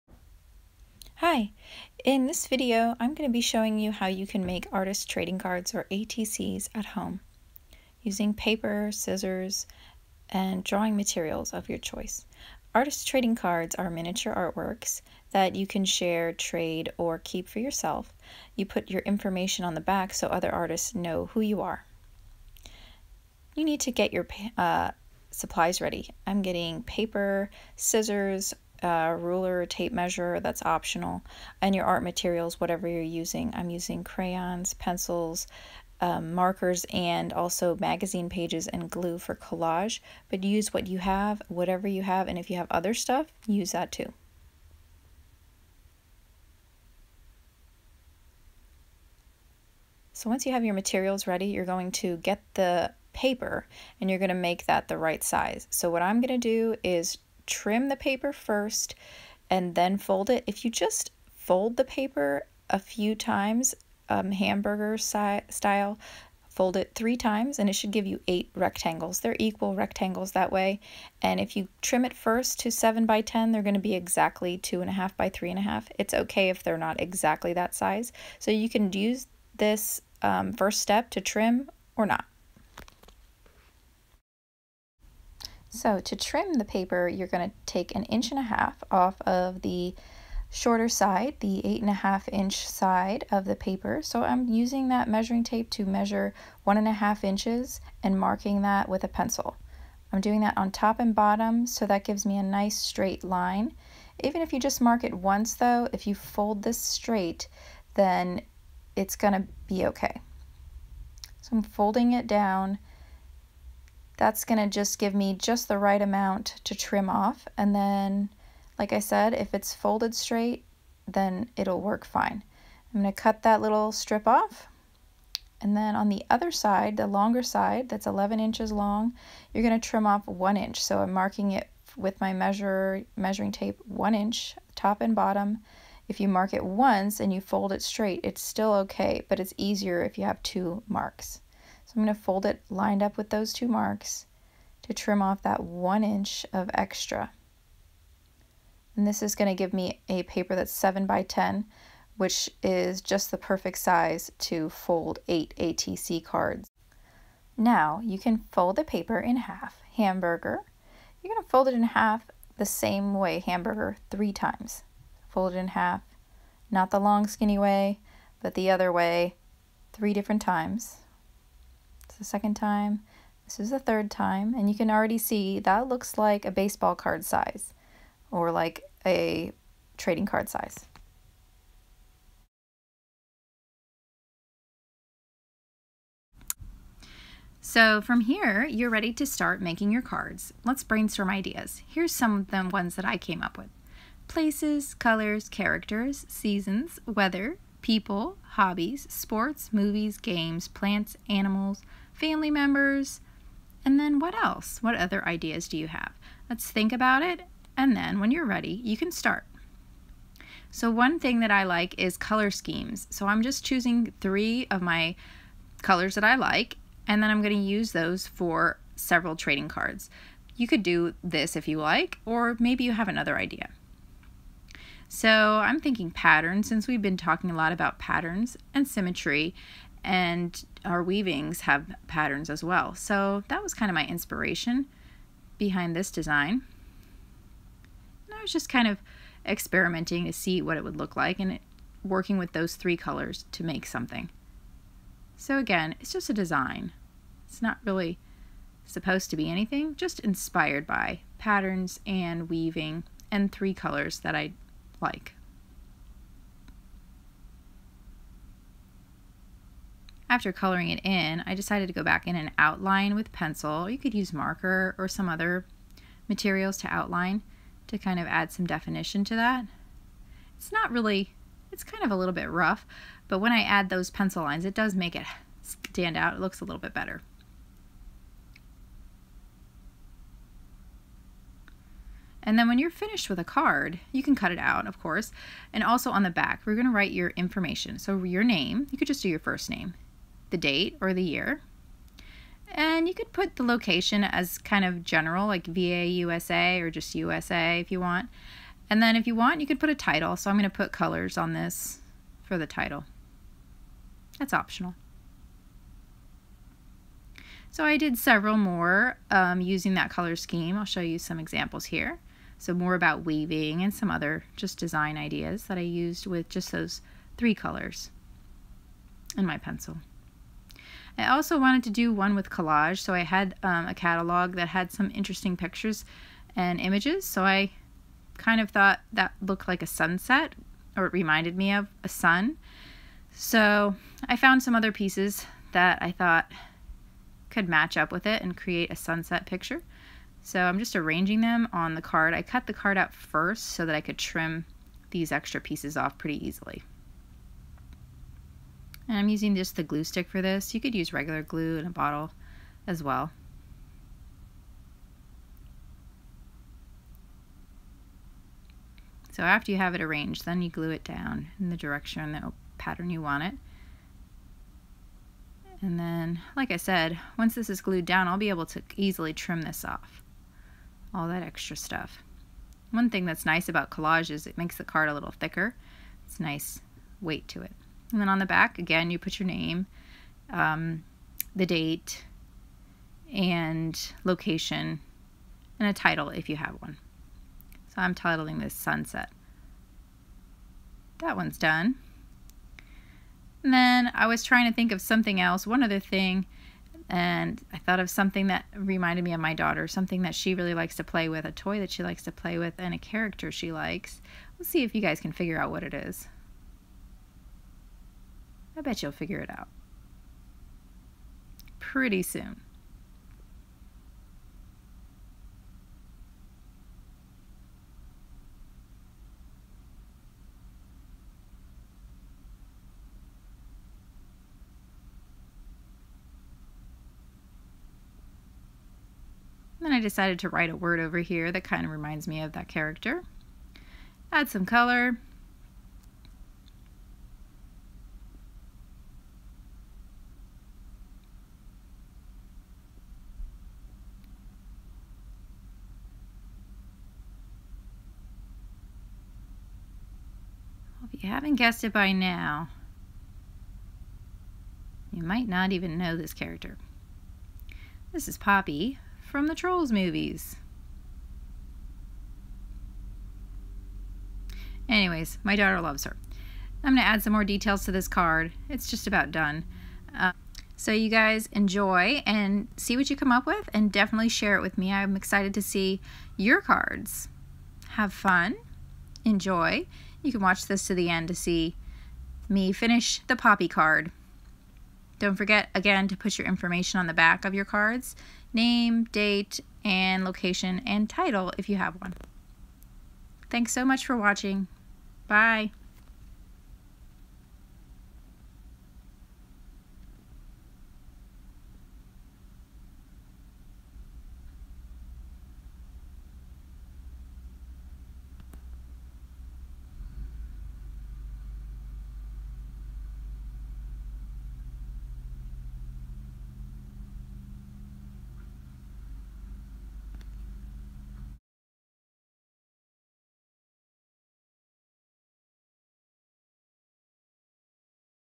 Making ATC’s at home
Use paper and scissors and any other art materials to make some artist trading cards along with me in this video!